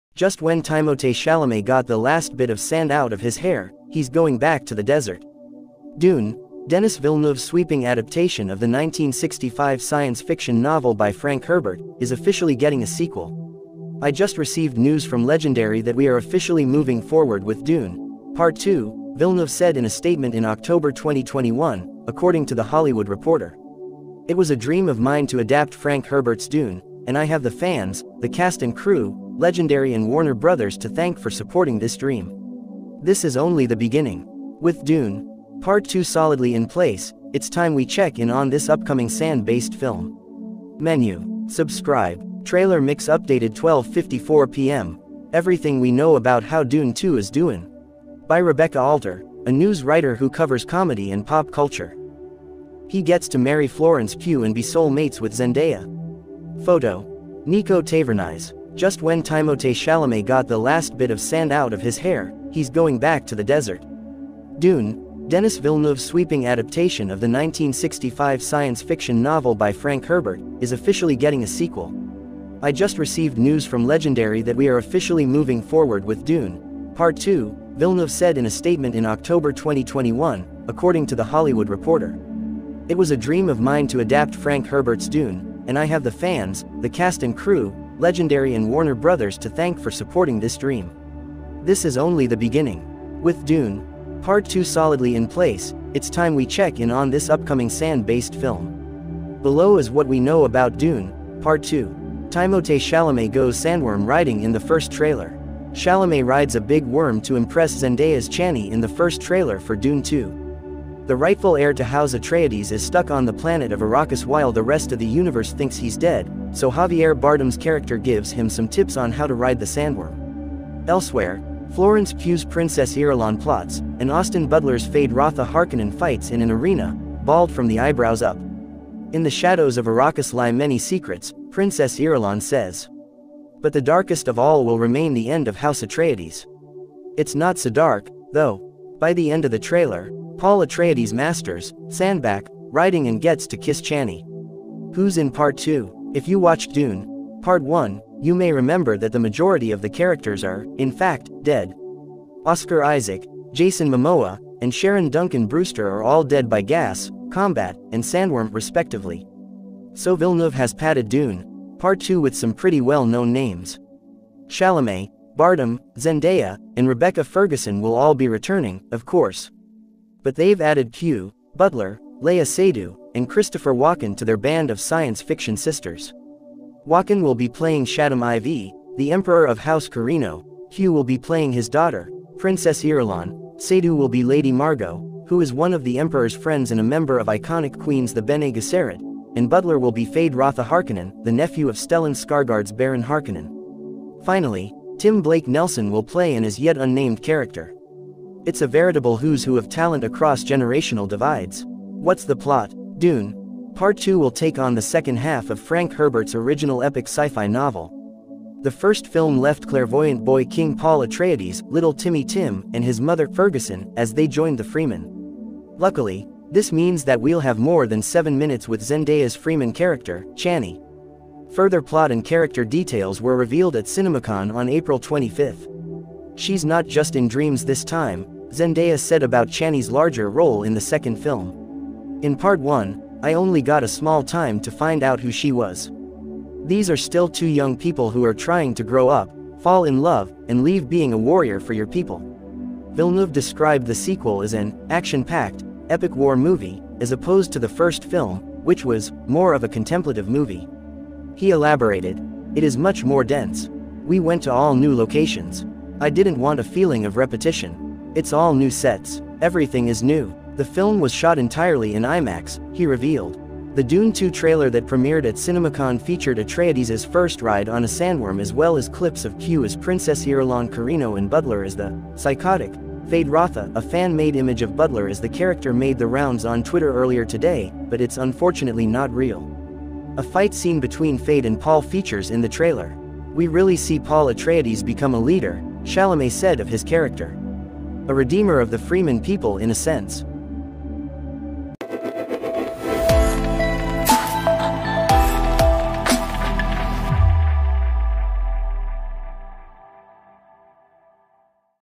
The Love Of Life And The Fate Of The Universe | Dune Part 2 | Movie Review
Paul Atreides joins the Chani and the Fremen in seeking revenge against the conspirators who destroyed his family. Faced with a choice between the love of his life and the fate of the universe, he must avoid a terrifying future that only he can foresee.

"Help us thrive! Your support fuels our growth and creativity."

Link have pin comment